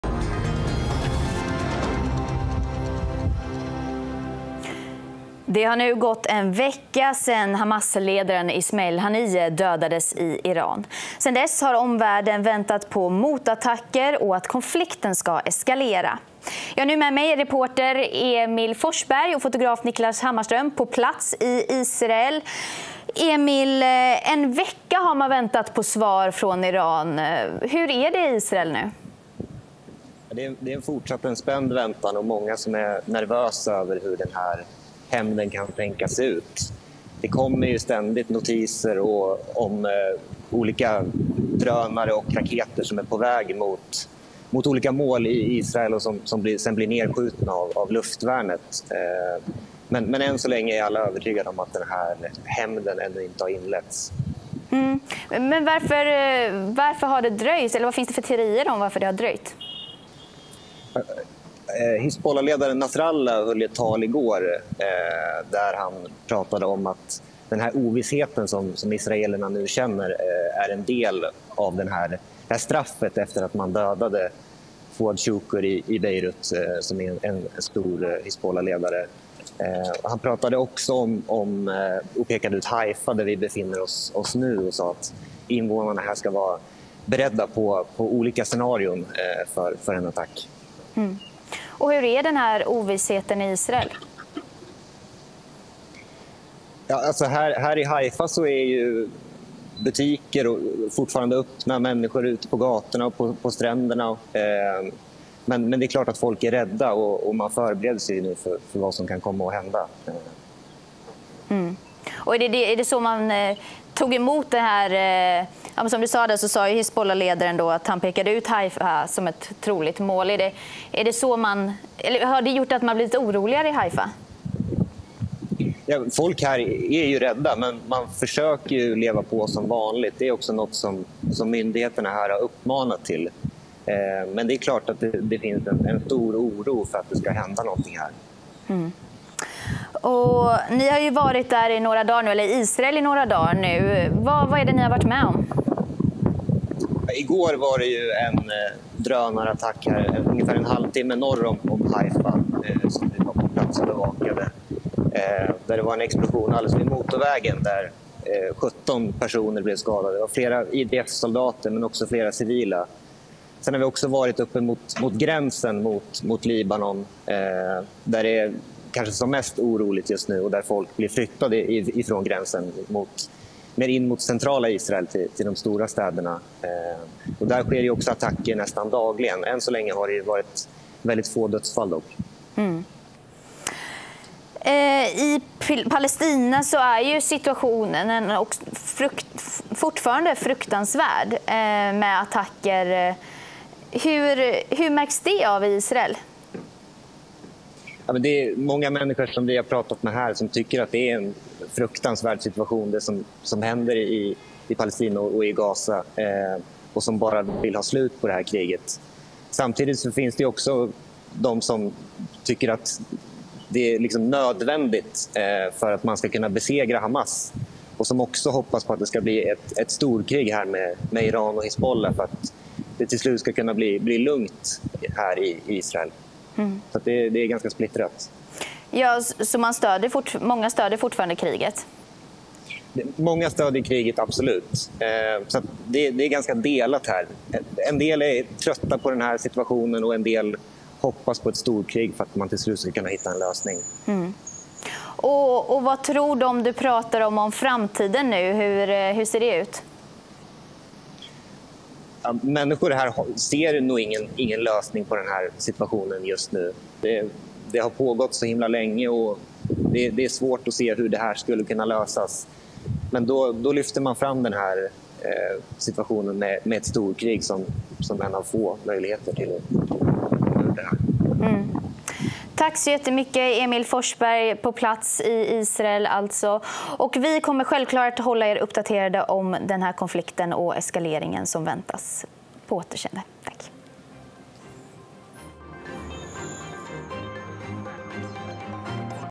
Israel väntar på hämndattacker – Aftonbladet på plats i Haifa
En vecka sedan Hamasledaren  Ismail Haniya dödades i Iran.
Hör Aftonbladets reporter på plats i Israel om det spända läget i området.